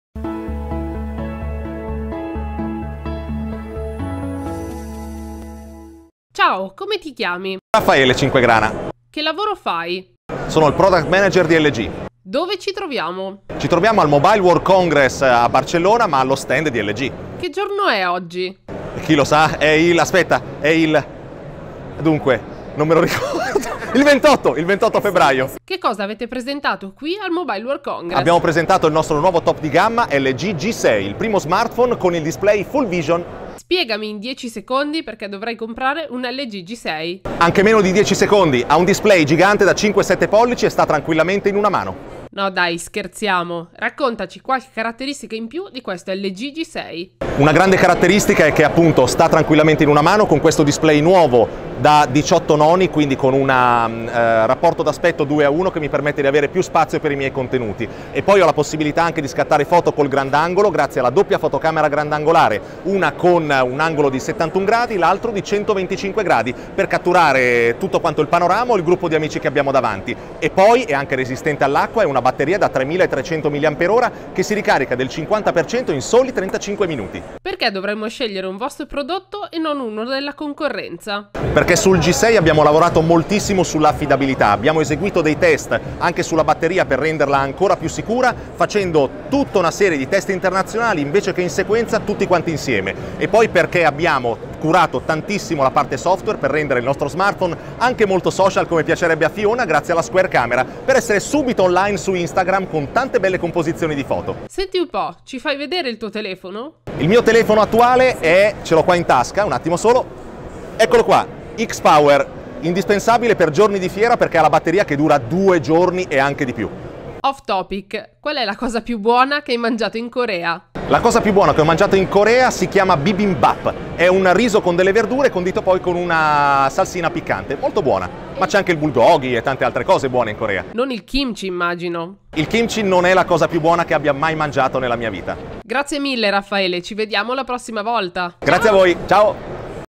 [LG] Raffaele Cinquegrana: perché dovremmo comprare LG G6?
Durante il Mobile World Congress di Barcellona abbiamo scambiato quattro simpatiche chiacchiere con Raffaele Cinquegrana, Product Manager di LG Italia. Tra le molte domande gli abbiamo chiesto perché dovremmo comprare un LG G6. Ecco la nostra divertente intervista.